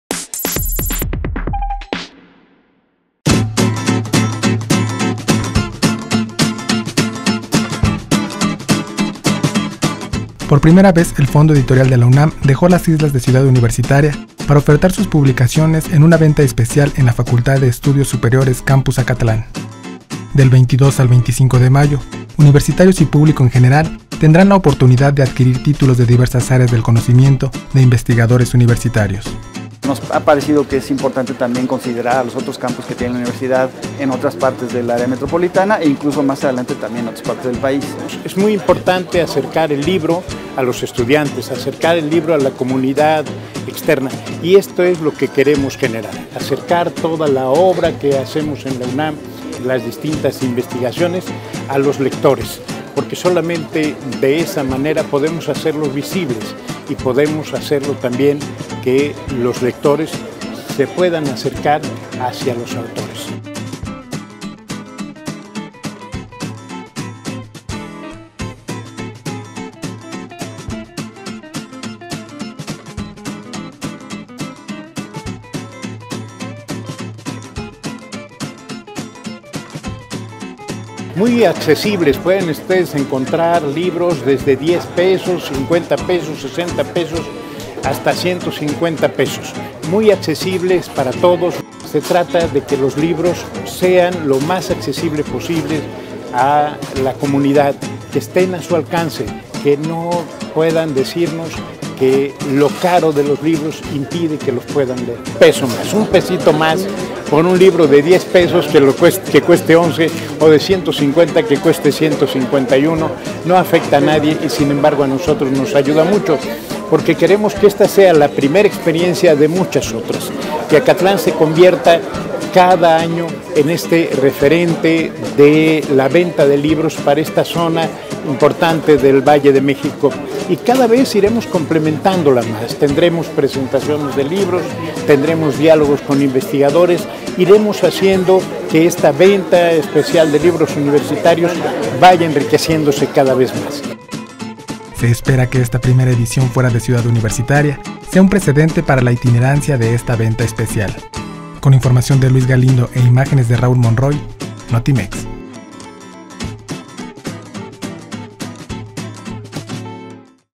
Por primera vez, venta especial de libros de la UNAM en FES Acatlán
Del 22 al 25 de mayo estudiantes y público en general podrán adquirir diversos títulos de investigadores universitarios a precios que van de los 10 a los 150 pesos.